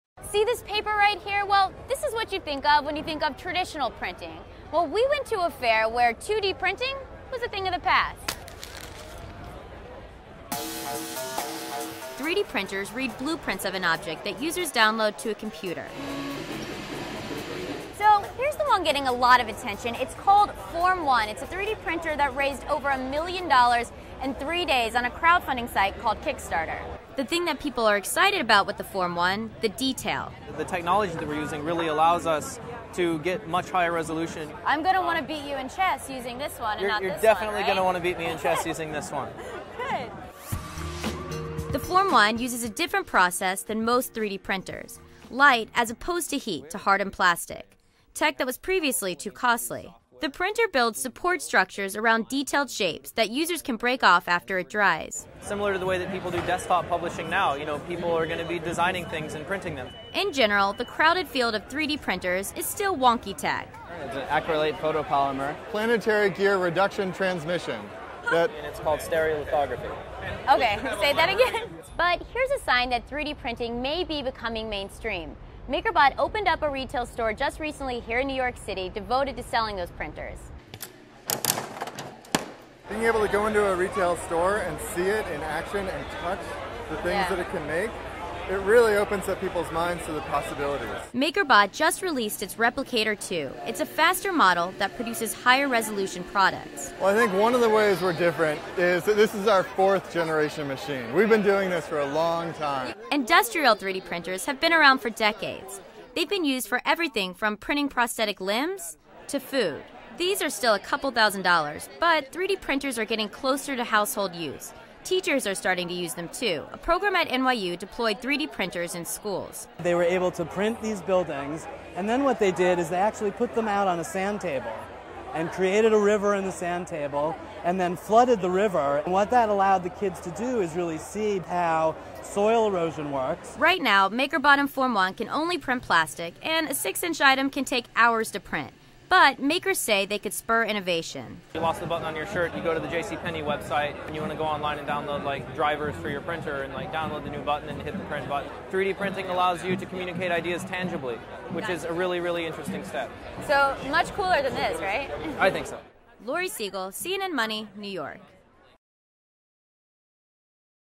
3D printing buttons, jewelry and shoes
3D printers are becoming more powerful and more mainstream, with a retail store opening up to sell them.